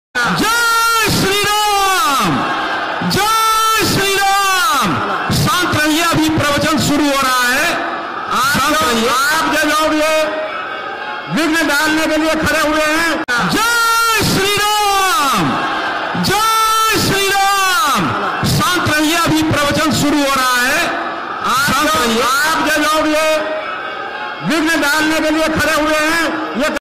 Dharmasansad - Tell us by when Ram mandir will be complete, sadhus angry on Mohan Bhagwat
धर्मसंसद - राम मंदिर कधी होणार तारीख सांगा, मोहन भागवतांवर साधू संतापले